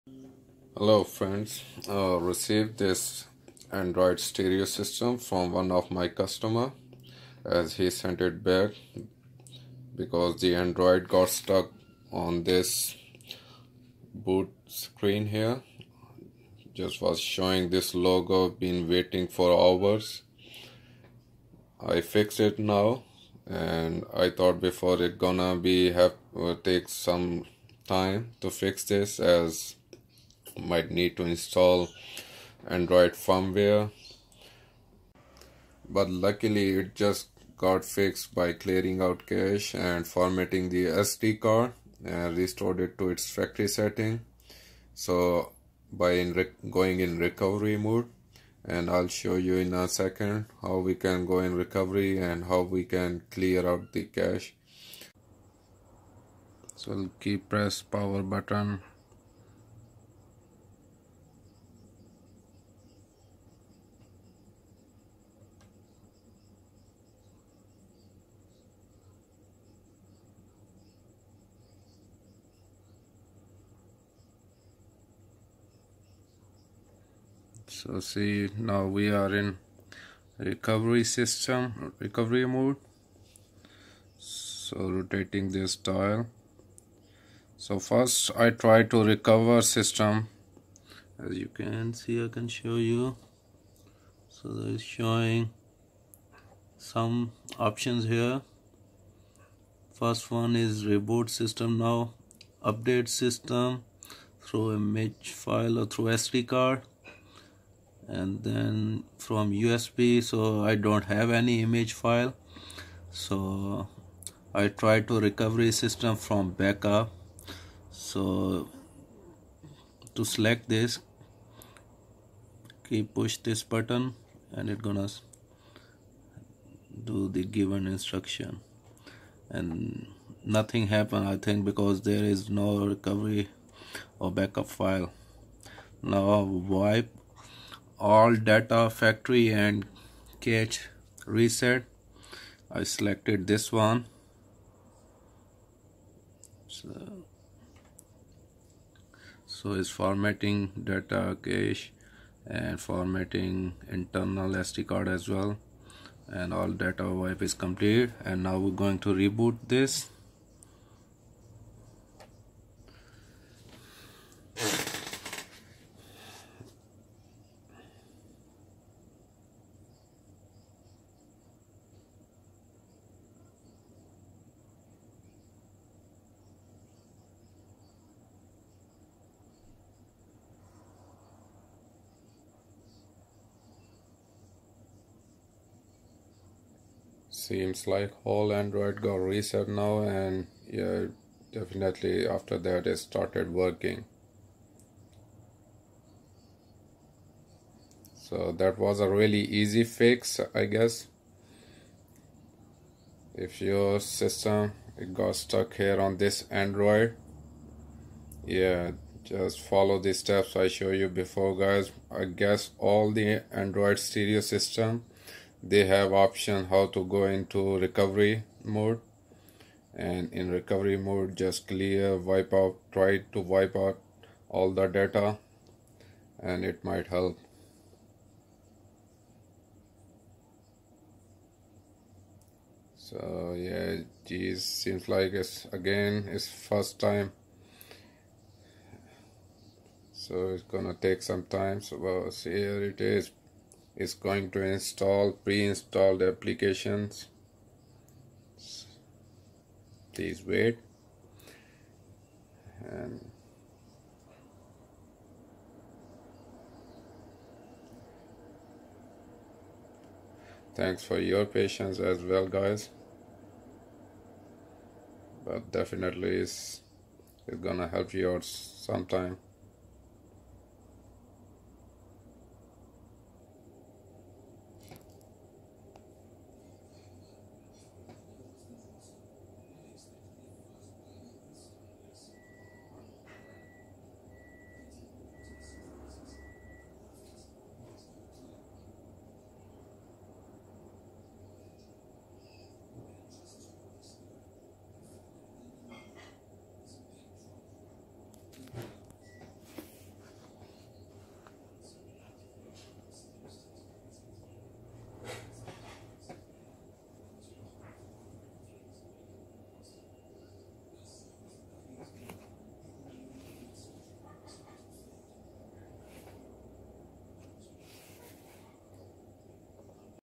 Android stereo stuck on boot screen fix, without reinstall firmware
This video help you to fix this boot screen stuck issue by using recovery mode and if you have no firmware or backup